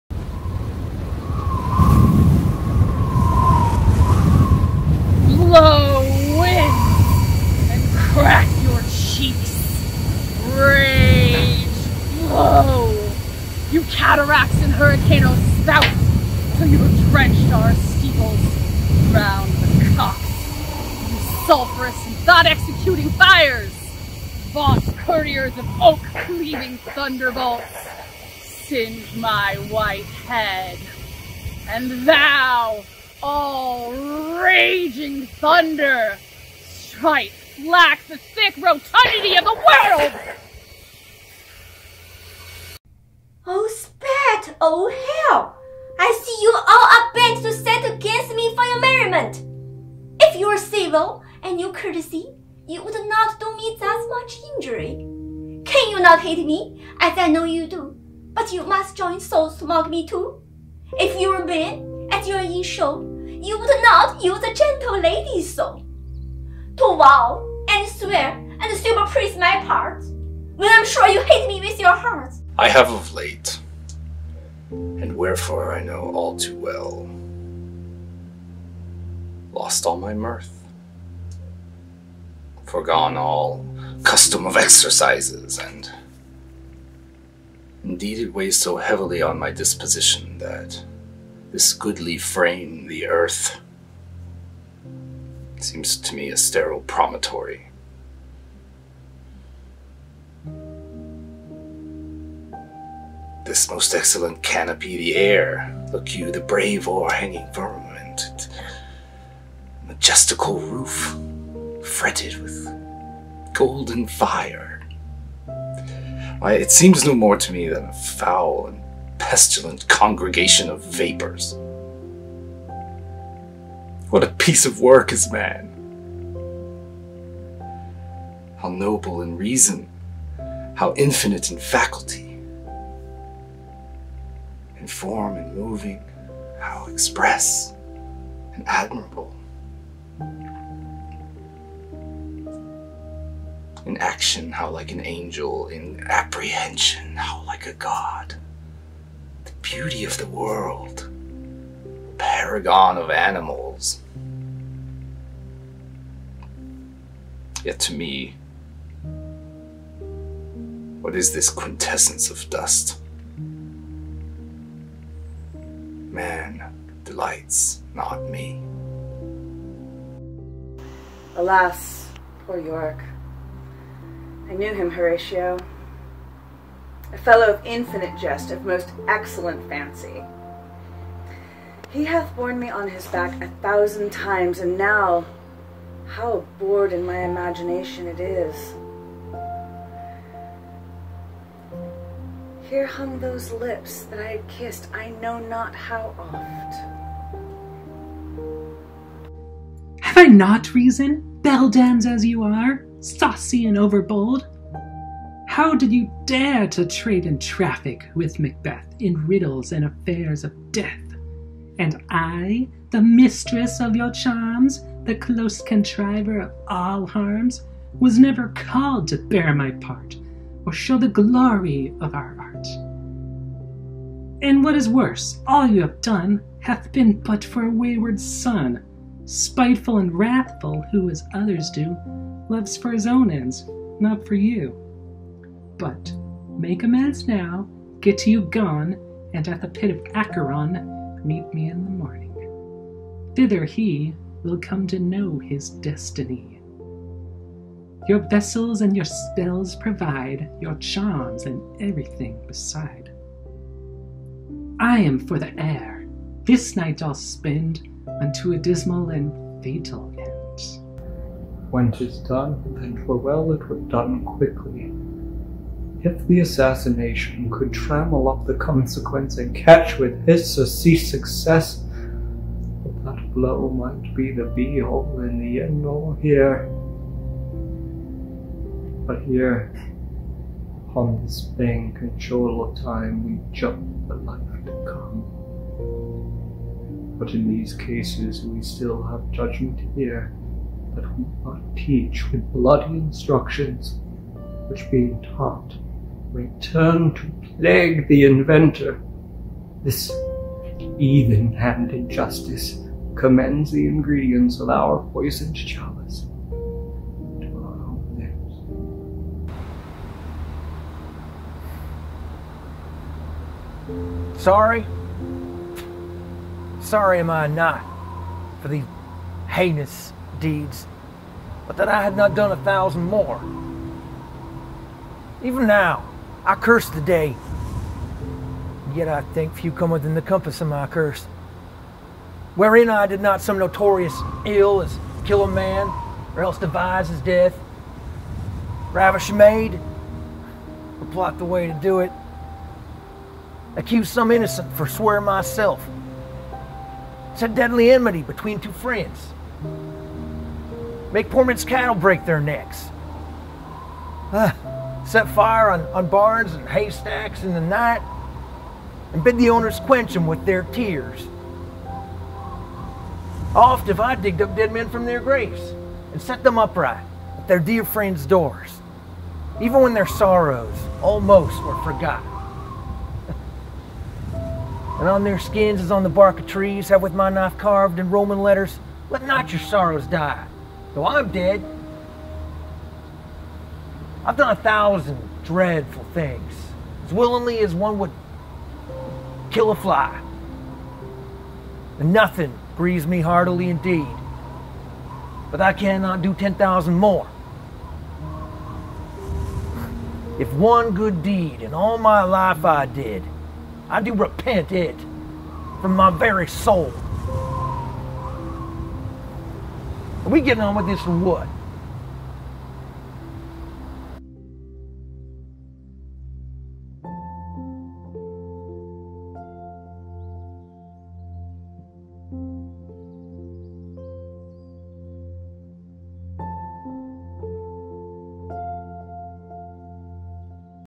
Shakespeare At Home Project
A challenge wherein people were asked to perform and submit a favorite piece of Shakespeare , with the goal of entertaining and bringing us a bit closer together even in our distance.